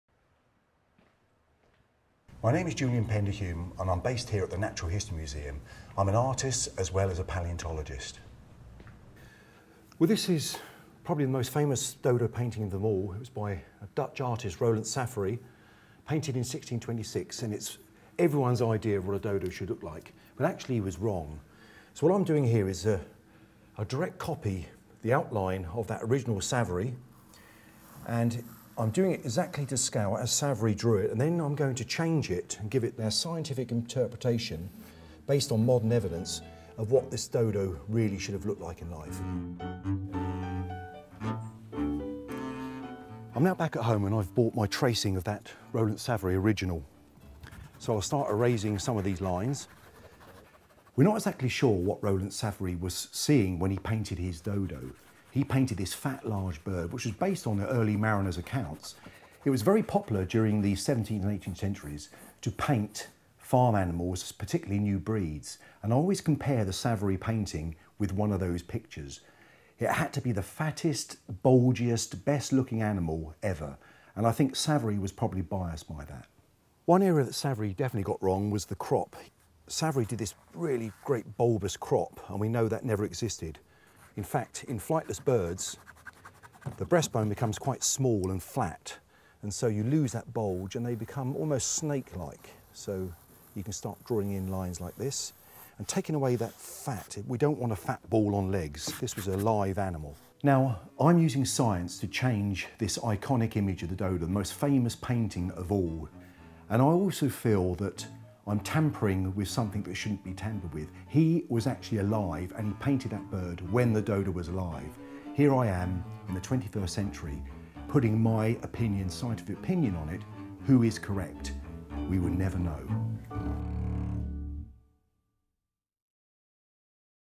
Dodo man deflates decades old myth | Images of Nature | Natural History Museum
The iconic dodo was given a makeover by palaeontologist and artist, Dr Julian Pender Hume as preparation for the opening of the Images of Nature gallery at the Museum.